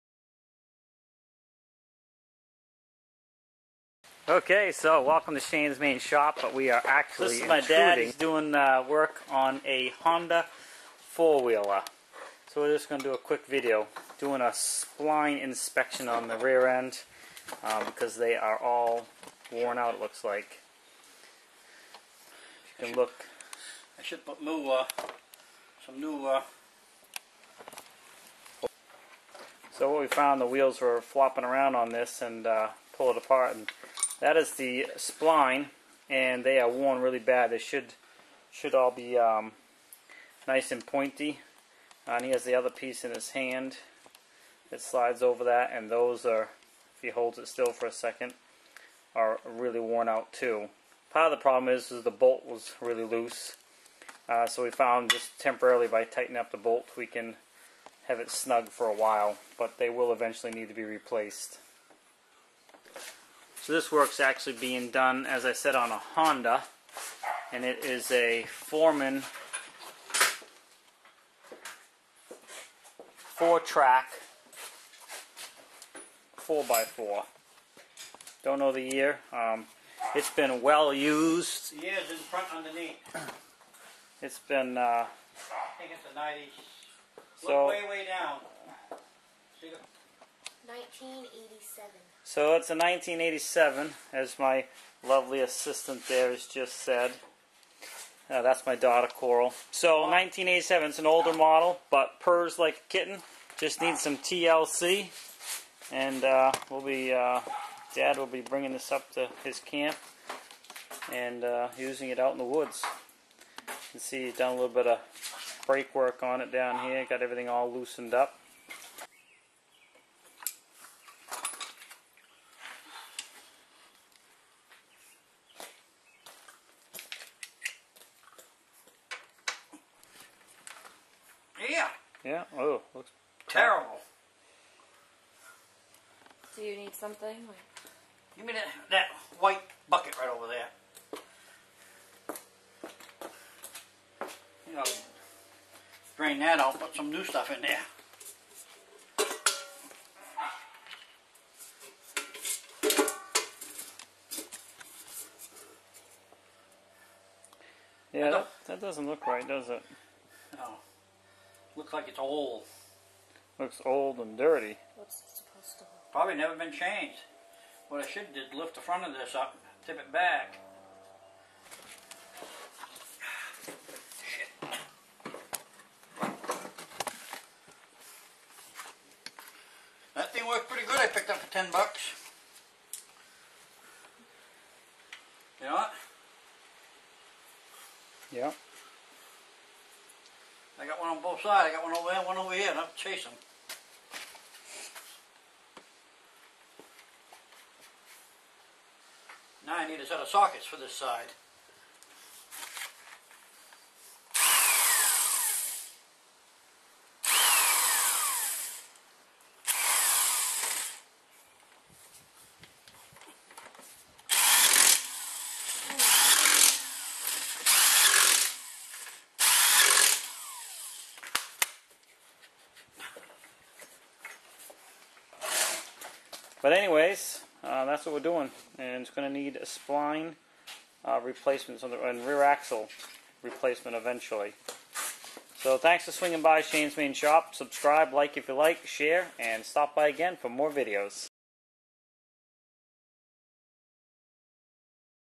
honda fourman fourtrax 4x4 rear axle check worn out spline
Repair work on a honda four wheeler breaks axle oil spline inspection  this four wheeler has a bad axle spline the spline is wore out. from to much mudding ill bet. shanesmaineshop